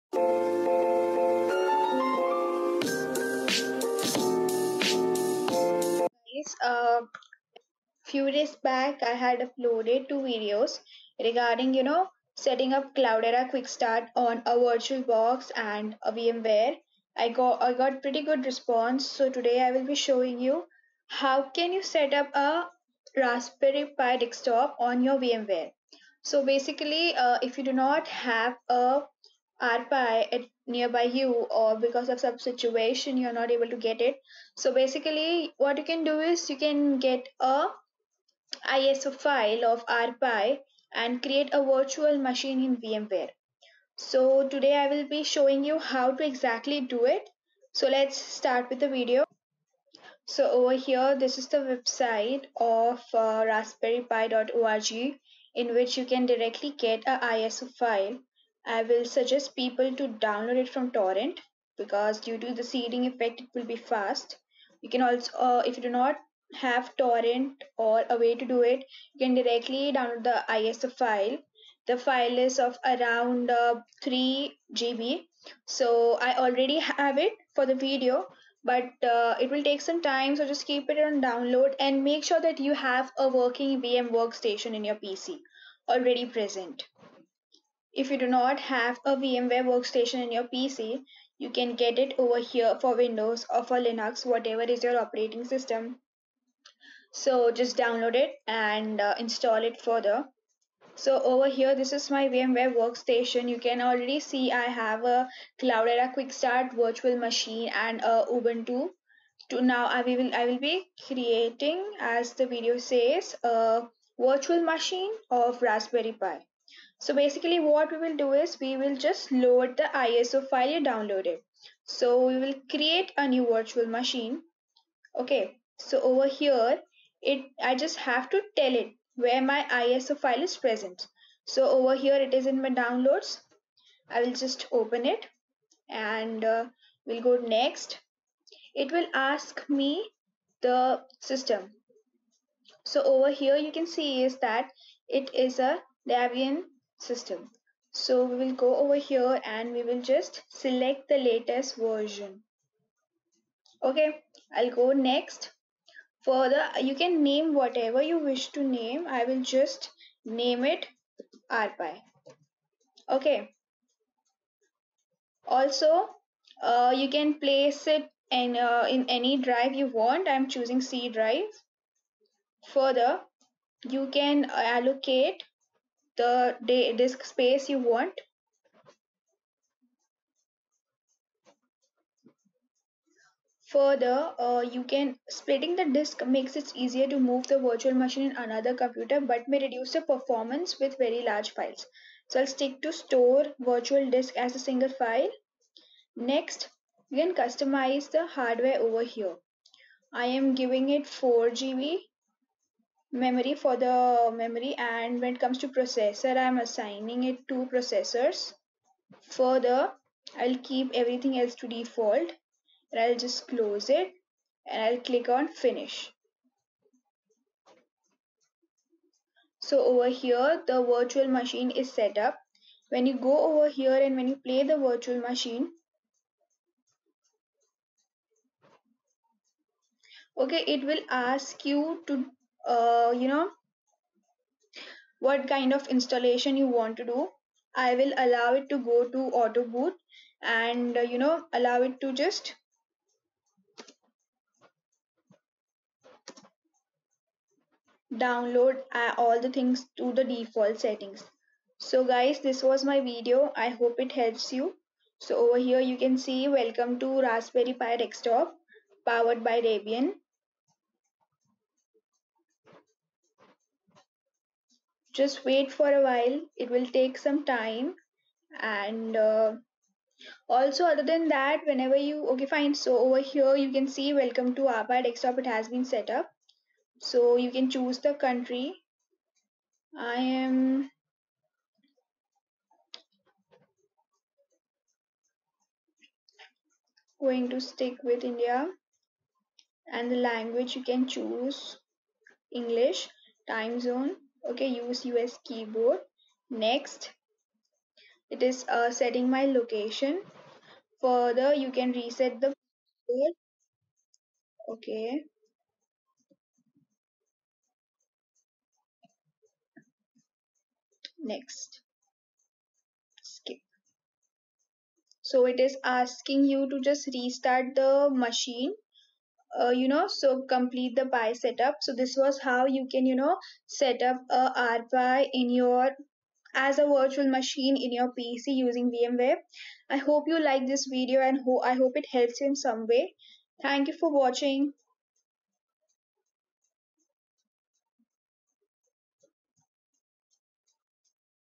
Download and install Raspberry Pi Desktop on vmware | raspberry pi without hardware
if you do not have Raspberry Pi hardware with no do not worry you can still run ISO file. you can easily create a virtual machine and make things work for you. I am setting up Rpi on vmware in this video. i hopw this helps.

0:00 – How to Set up Raspberry Pi Desktop on VMware
0:49 – Where to Download Raspberry Pi ISO for VMware
1:30 – Installing VMware Workstation on Windows/Linux
2:12 – How to Create a Virtual Machine on VMware
3:10 – Customize Virtual Machine Settings for Raspberry Pi
3:58 – How to Boot Raspberry Pi on VMware
4:59 – Complete Raspberry Pi Setup in VMware